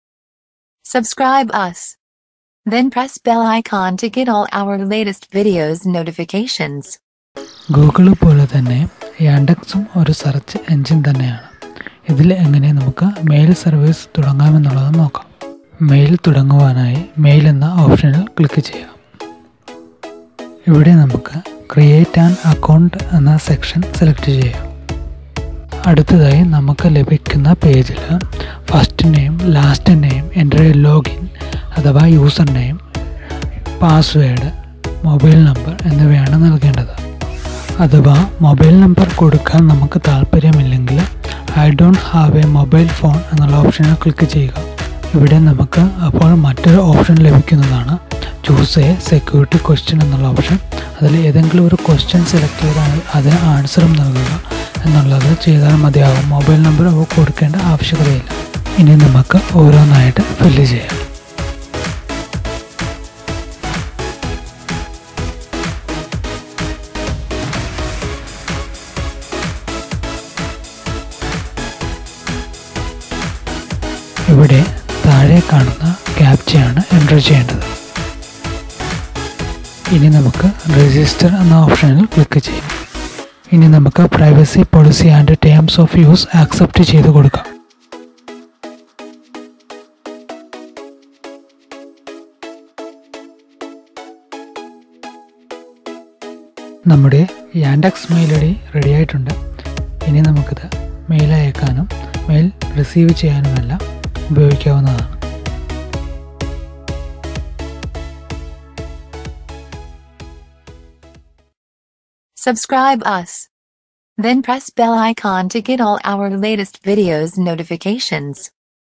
How to create a Yandex Mail | Like this - Gmail Alternative | Malayalam Tutorial
Here i am showing you that about - How to create a new Yandex Mail . It is very easy to create this account on Yandex. This is a Malayalam Tutorial about Yandex Mail.